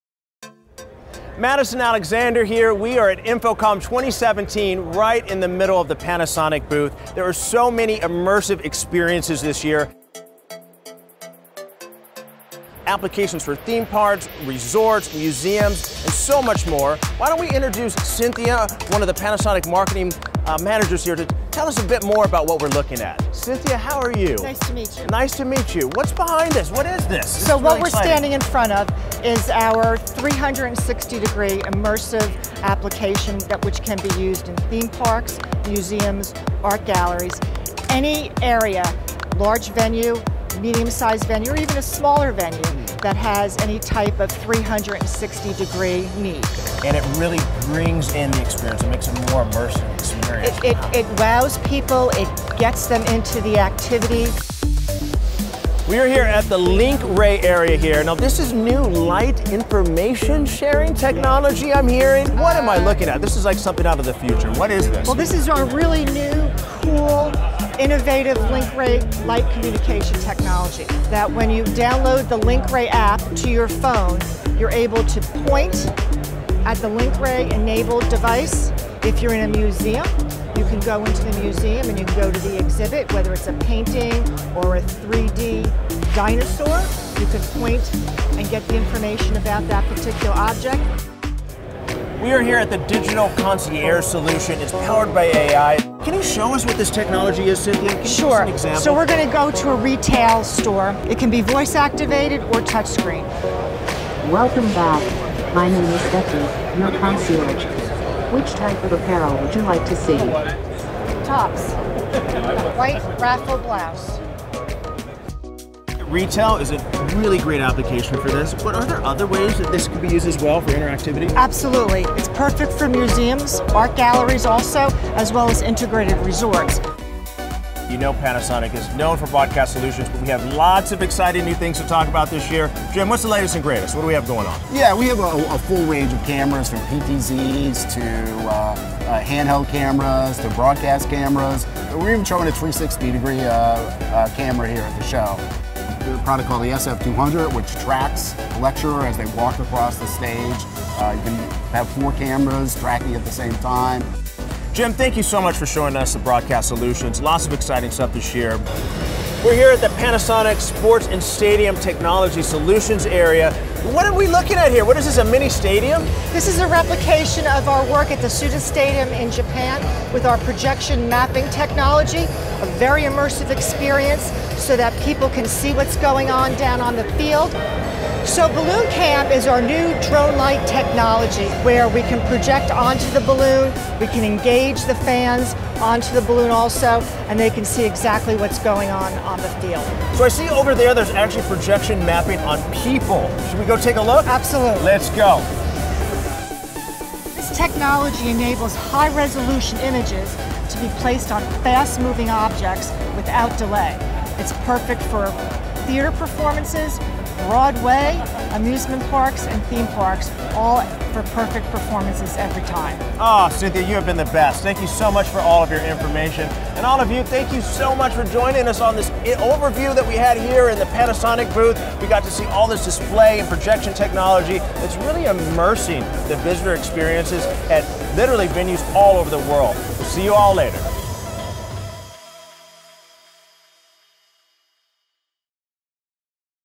Panasonic Showcases Next-Gen Tech At InfoComm 2017
Live from the show floor at InfoComm 2017: See how Panasonic is using cutting-edge technology to transform the way you experience live events. From High-Resolution fish-eye lens in a 360˚ dome to projection mapping, we are making the magic happen. AI, digital concierge, and other emerging technology are why some of the biggest brands are choosing Panasonic to help tell stories that connect with their audiences.